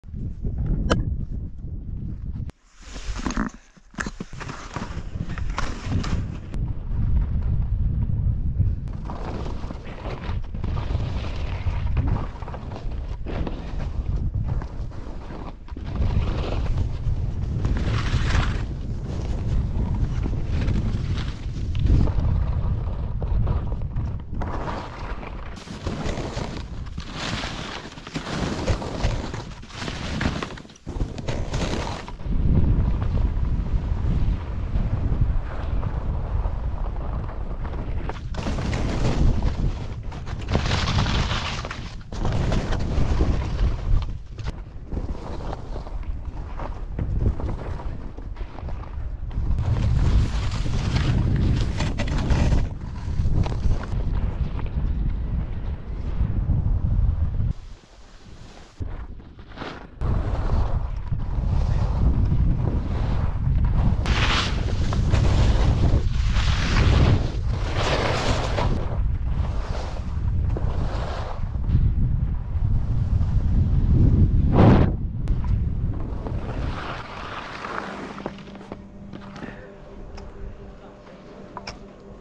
I went Telemark Skiing Instead of Working
A nice Friday telemark skiing in Loveland Ski Resort in Colorado. Lift #8.

More sailing videos coming soon...